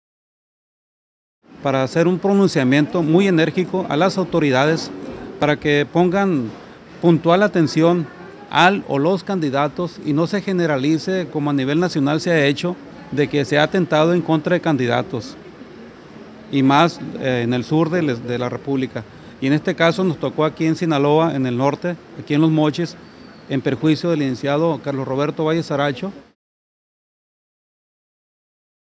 Exigen se aclare agresión contra abogado y candidato a regidor
La Federación de Colegios de Asociaciones de Abogados piden investigación por agresión en contra de Carlos Roberto Valle Saracho